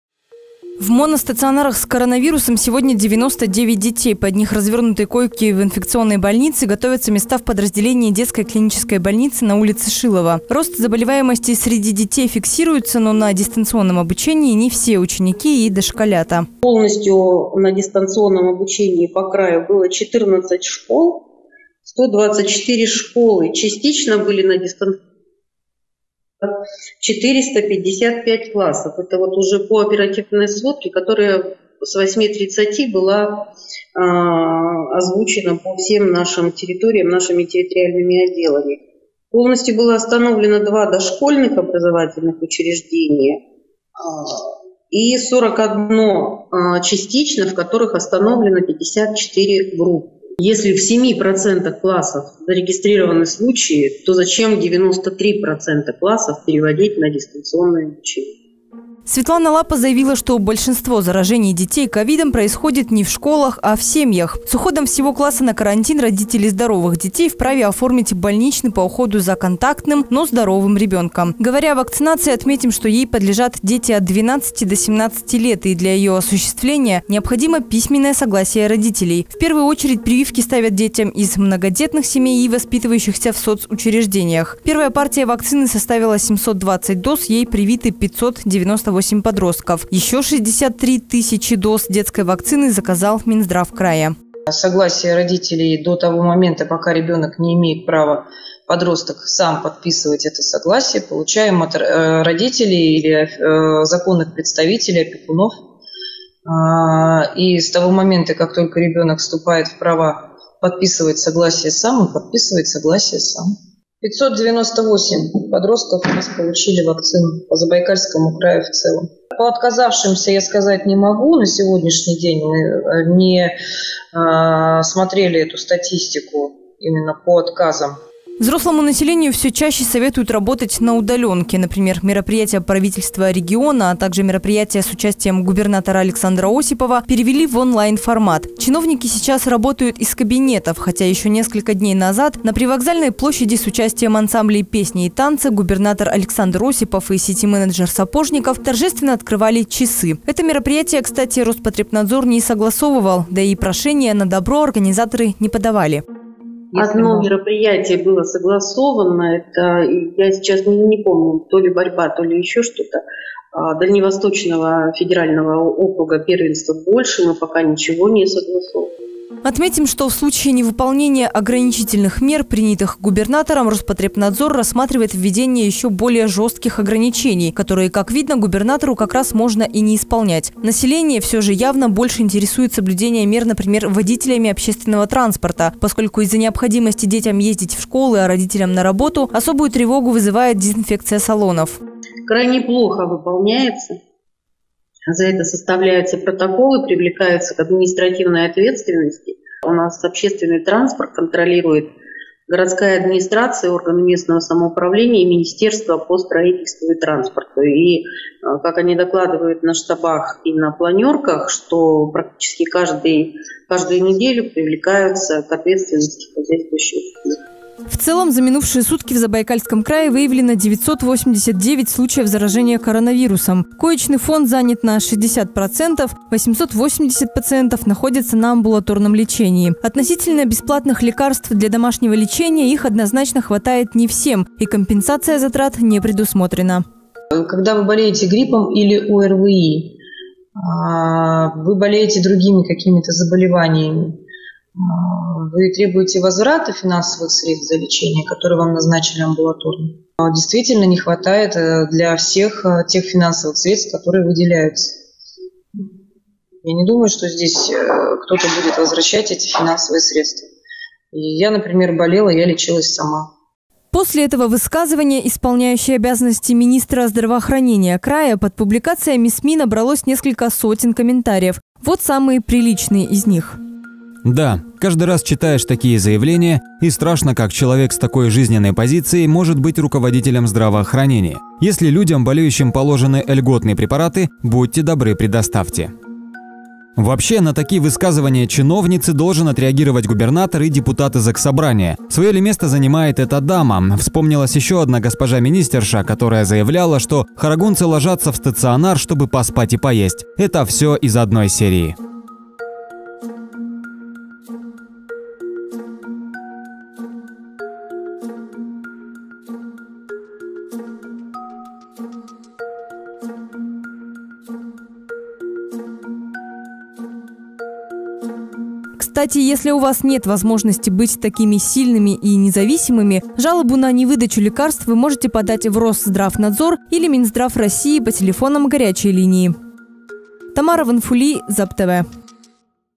Оксана Немакина о бесплатных лекарствах от COVID-19: «Я лечилась сама»
Сильная и независимая - и.о. министра здравоохранения региона на вопрос о нехватке бесплатных лекарств для амбулаторного лечения коронавируса, заявила, что когда болела, покупала препараты за свой счёт. Такое высказывание вызвало бурю негодования среди забайкальцев. Лекарства, вакцина для детей, дистанционное обучение и другие темы пандемии в материале моих коллег.